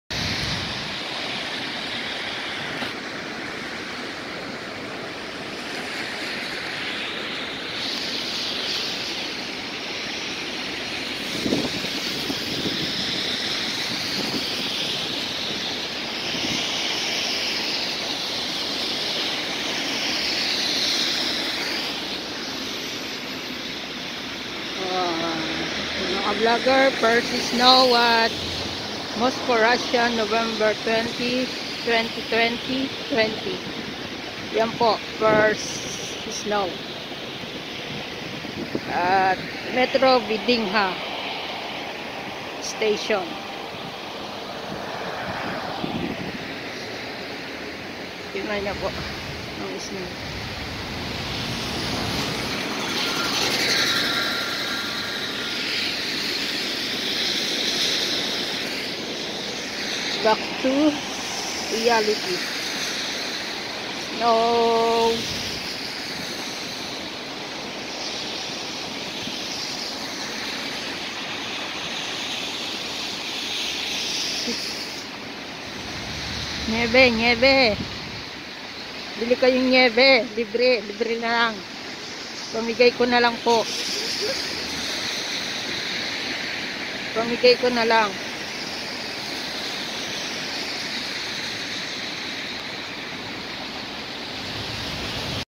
First Snow( Moscow)
1st Snow Here At Moscow
winter season 2020
near at metro vdnkh